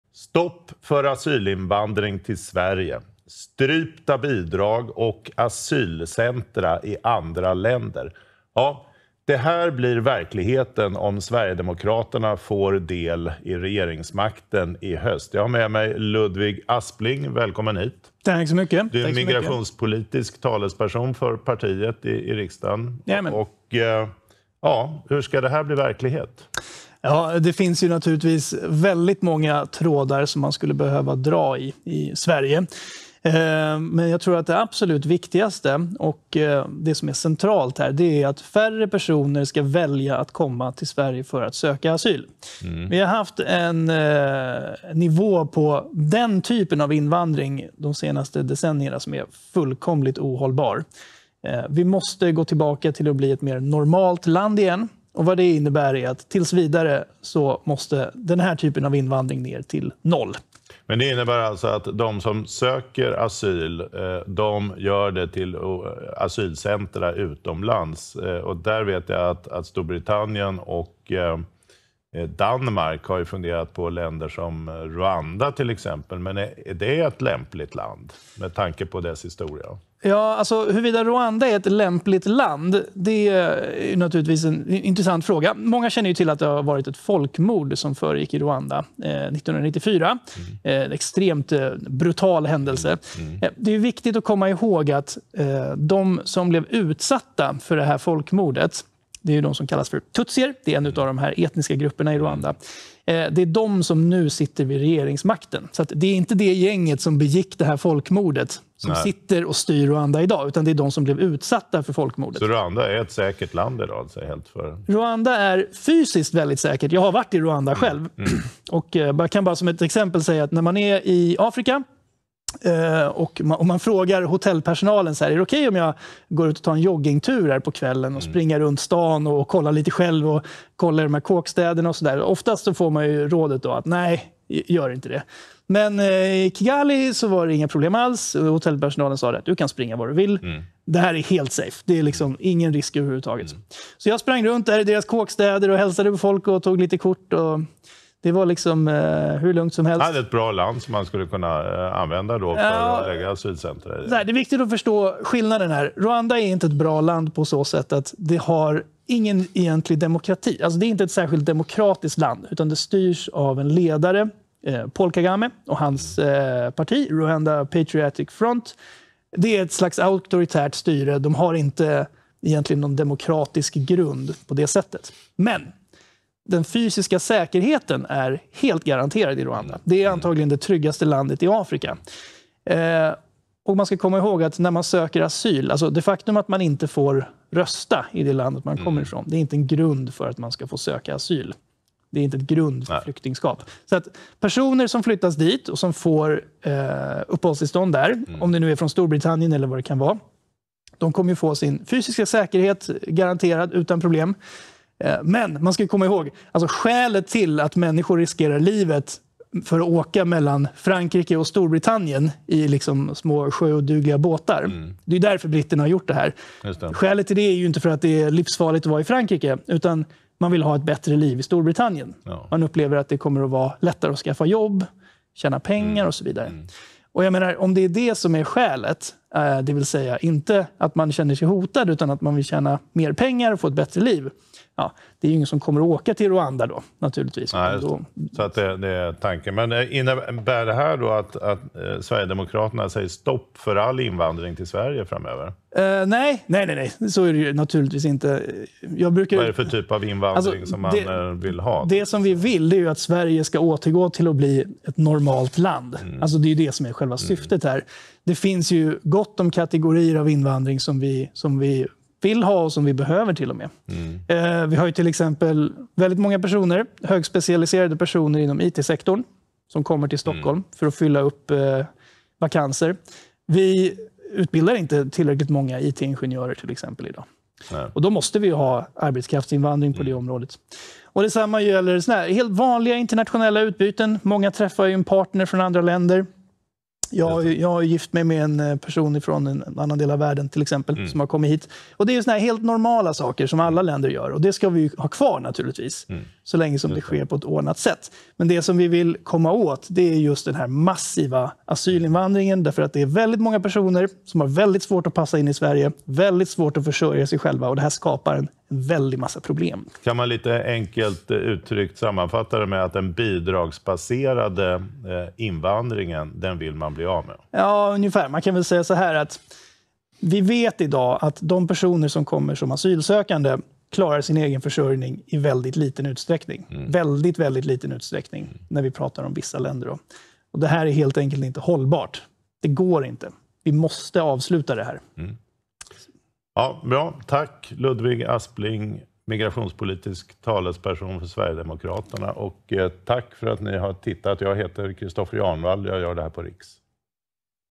SD: Så här stoppar vi invandringen till Sverige | Intervju med Ludvig Aspling
Riks intervjuar Ludvig Aspling, migrationspolitisk talesperson för Sverigedemokraterna om några av de förslag som partiet har för att få ner invandringen till Sverige till en miniminivå.